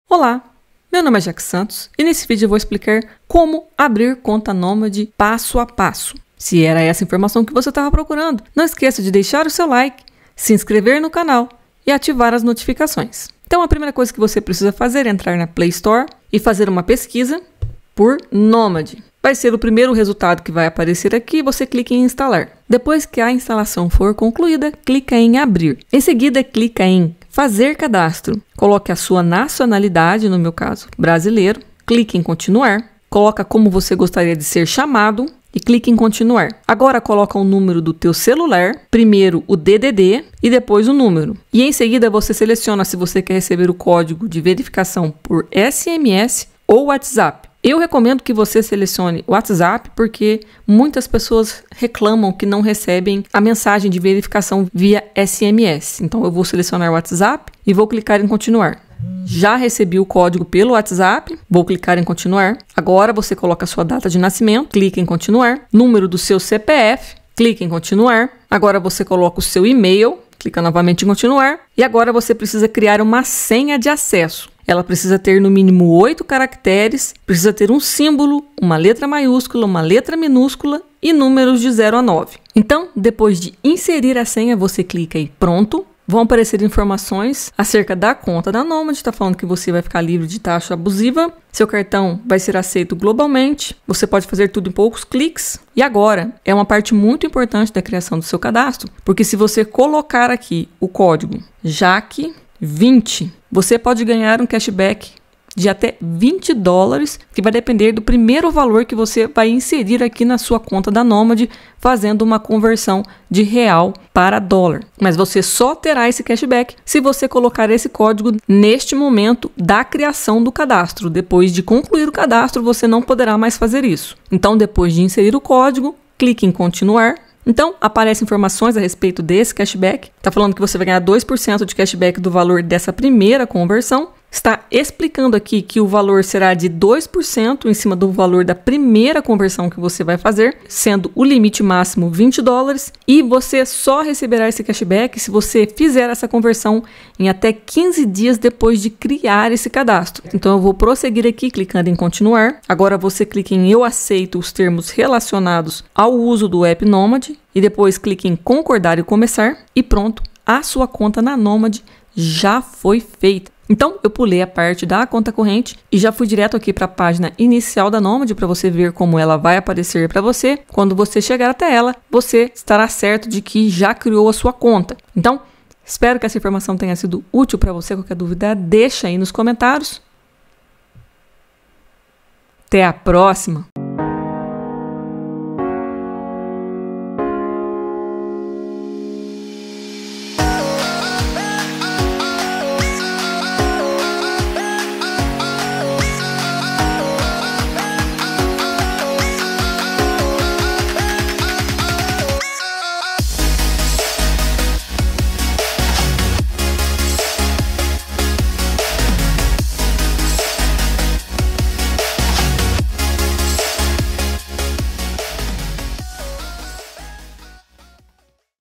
Nomad Como Abrir Conta Passo a Passo 2024 (cadastro com cashback)
✅ Use o código: JACKIE20 para ganhar até U$20 de cashback.
Ele dá um cashback de 2% em cima da primeira operação de câmbio realizada em 15 dias desde que o código foi inserido no app. O valor máximo de cashback é de US$ 20.